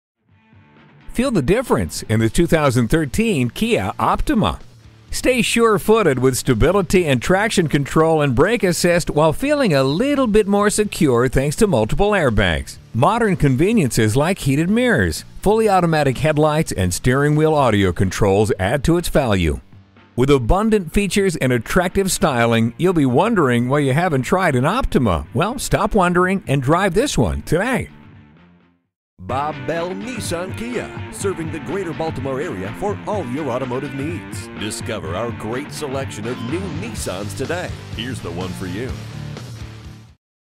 2013 Kia Optima Baltimore MD Dundalk, MD 23234Z - SOLD
Year: 2013
Make: Kia
Model: Optima
Trim: LX
Engine: 4 Cyl - 2.4L
Transmission: Automatic
Color: Satin Metal Metallic
Interior: Gray w/Cloth Seat Trim
Mileage: 42898
Stock #: 23234Z
VIN: 5XXGM4A76DG225291

7900 Eastern Ave.

Address:
7900 Eastern Ave